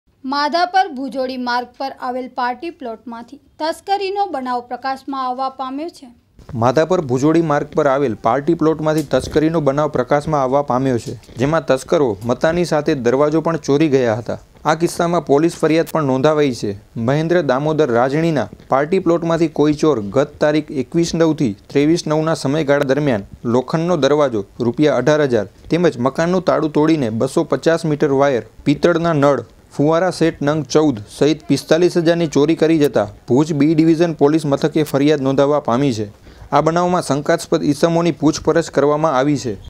માધાપર-ભૂજોડી માર્ગ પર આવેલ પાર્ટીપ્લોટ માંથી તસ્કરીનો બનાવ પ્રકાશમાં આવવા પામ્યો છે..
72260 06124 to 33